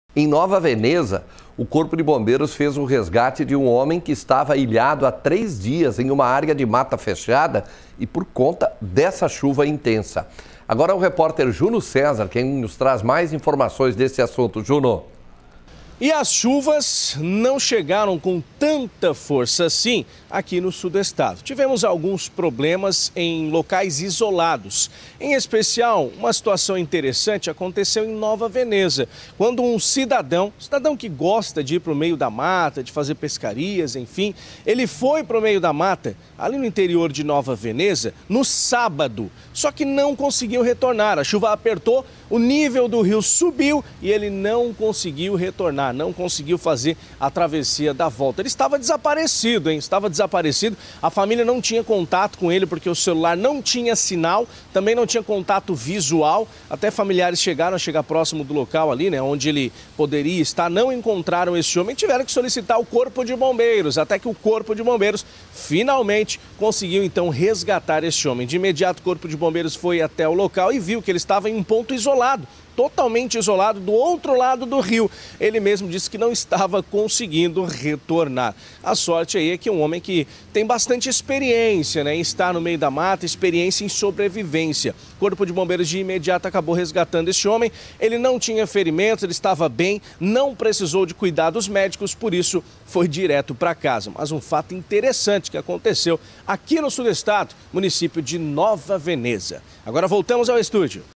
Corpo de Bombeiros resgata homem ilhado por três dias em área de mata fechada em Nova Veneza
Em Nova Veneza, o Corpo de Bombeiros fez o resgate de um homem, que estava ilhado há três dias em uma área de mata fechada, por conta da chuva intensa.